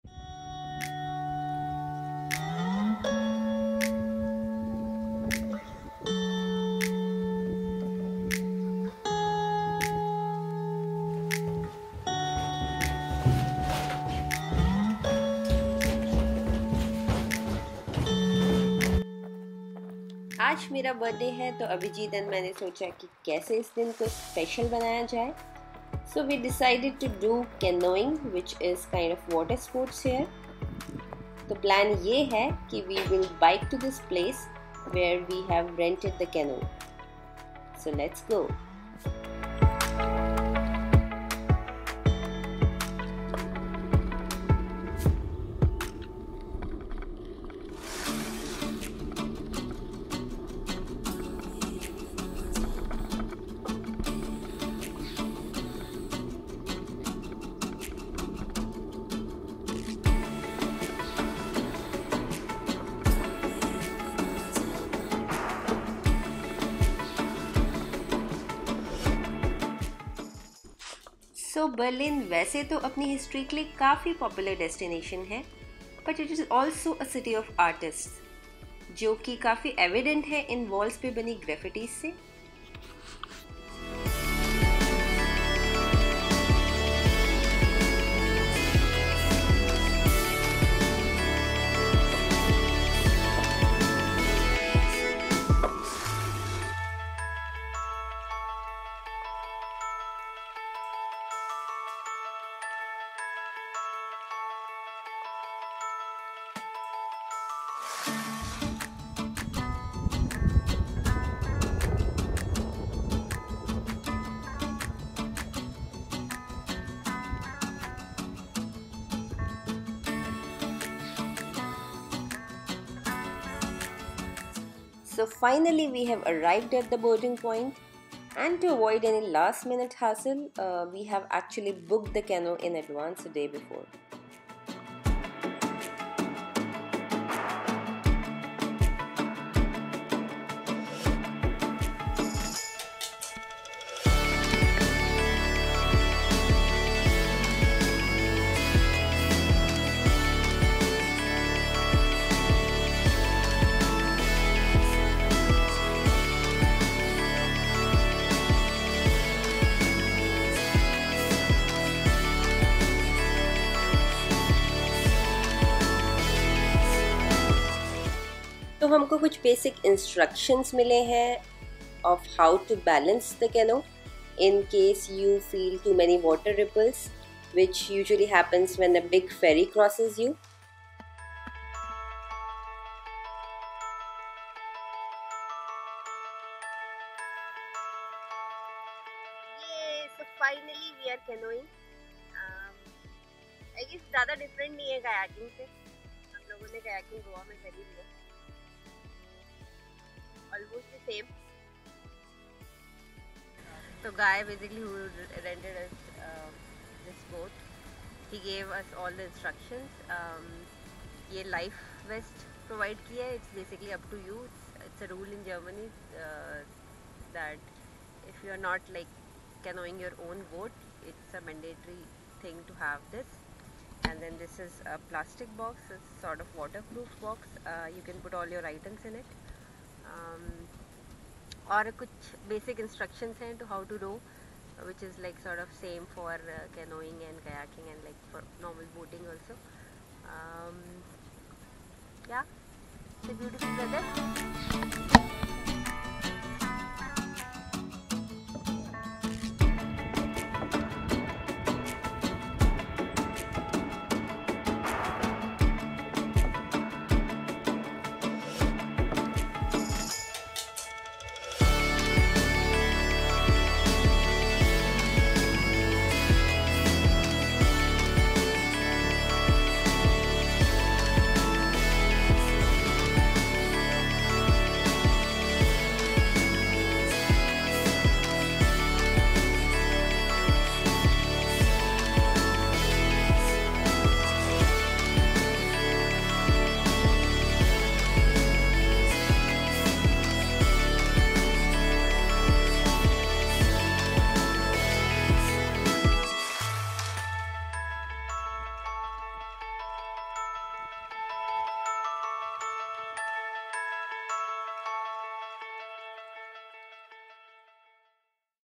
Our First Canoeing Experience In Berlin | Summer Fun | Spree River | Treptow Park
Hey Lovely people, sharing our first ever canoeing experience this summer with you all !!!